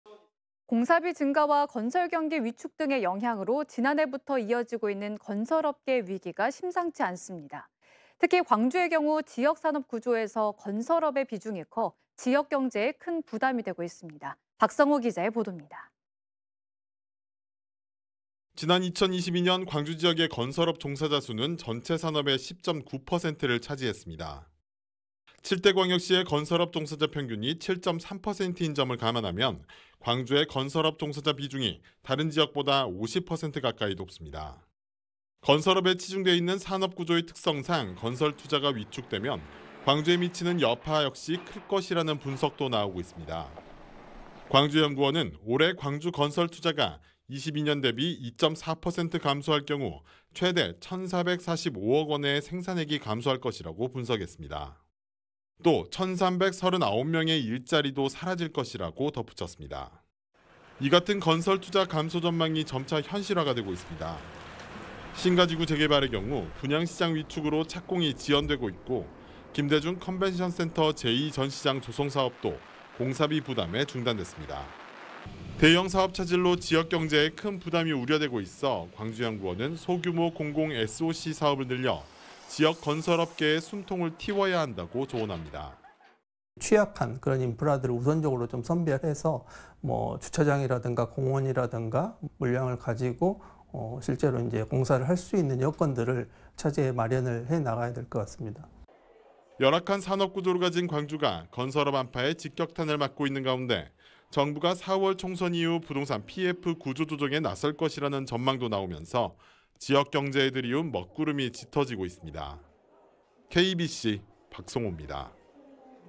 건설업 비중 높은 광주, 건설경기 위축에 직격탄 / KBC뉴스
공사비 증가와 건설경기 위축 등의 영향으로 지난해부터 이어지고 있는 건설업계의 위기가 심상치 않습니다.

특히, 광주의 경우 지역 산업구조에서 건설업의 비중이 커 지역 경제에 큰 부담이 되고 있습니다.

박성호 기자의 보도입니다.